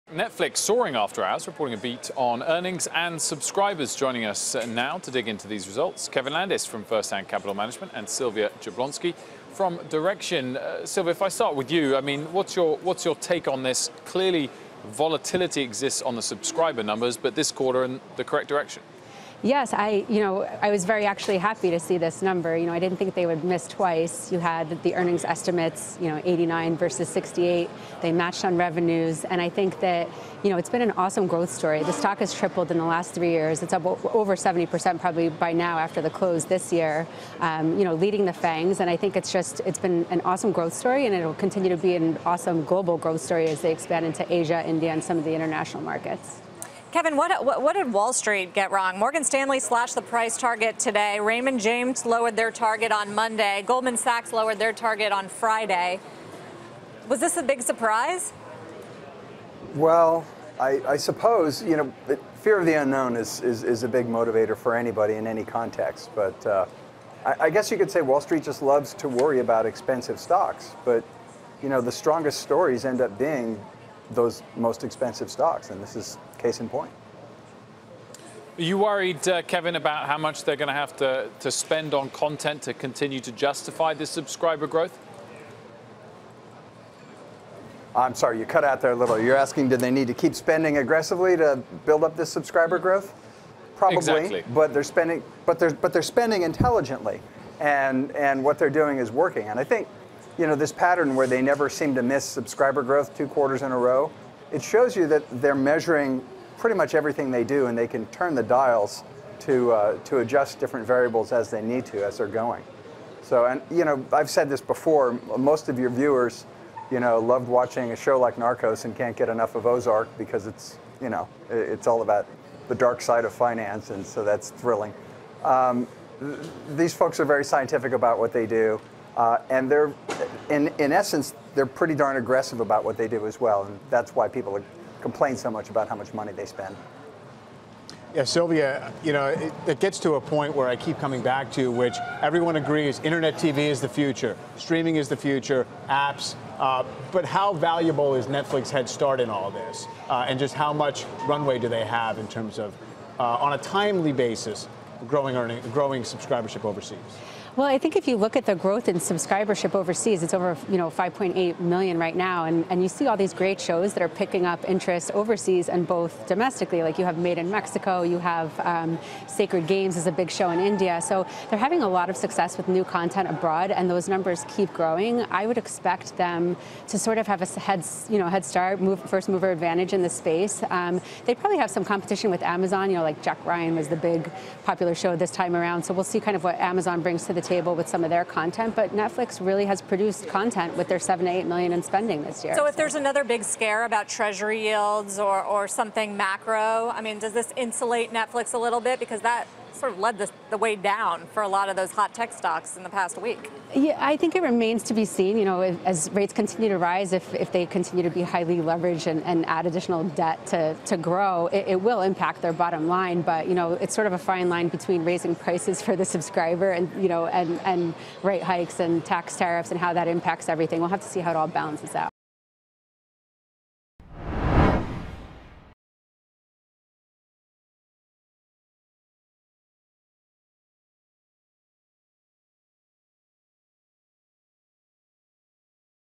Netflix spending 'intelligently' to boost subscriber growth: Strategist
Kevin Landis of Firsthand Capital Management and Sylvia Jablonski of Direxion discuss their outlook for Netflix after the company posted strong third-quarter earnings that prompted the stock to surge.